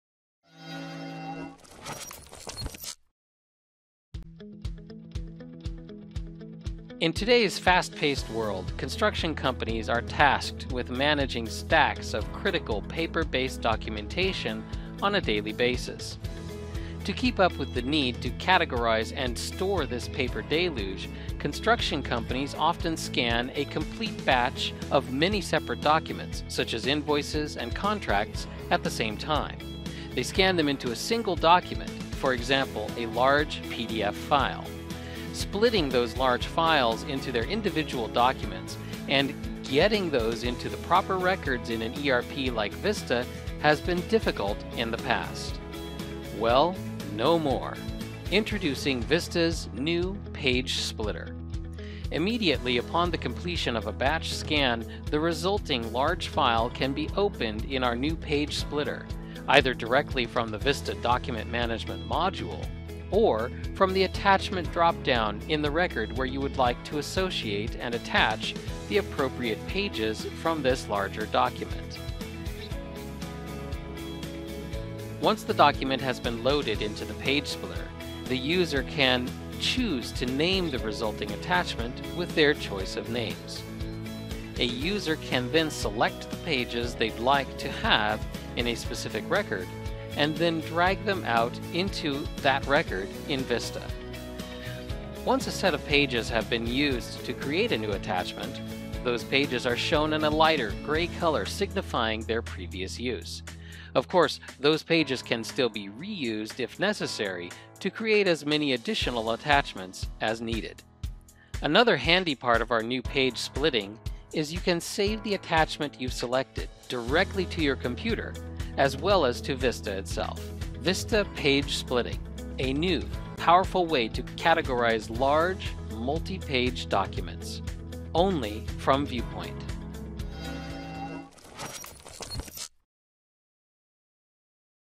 Document Scanning and Page Splitting
It’s never been easier to scan, split and organize documents in Vista.

"In today's fast paced world, construction companies are tasked with managing stacks of critical paper-based documentation on a daily basis. To keep up with the need to categorize and store this paper deluge, construction companies often scan a complete batch of many separate documents such as invoices and contracts at the same time. They scan them into a single document. For example, a large PDF file. Splitting those large files into their individual documents and getting those into the proper records in an ERP like Vista has been difficult in the past.

Well, no more. Introducing Vista's new page splitter. Immediately upon the completion of a batch scan, the resulting large file can be opened in our new page splitter either directly from the Vista document management module or from the attachment dropdown in the record where you would like to associate and attach the appropriate pages from this larger document.

Once the document has been loaded into the page splitter, the user can choose to name the resulting attachment with their choice of names. A user can then select the pages they'd like to have in a specific record and then drag them out into that record in Vista. Once a set of pages have been used to create a new attachment, those pages are shown in a lighter gray color signifying their previous use. Of course, those pages can still be reused if necessary to create as many additional attachments as needed. Another handy part of our new page splitting is you can save the attachment you've selected directly to your computer, as well as to Vista itself. Vista page splitting, a new powerful way to categorize large multi-page documents, only from Viewpoint."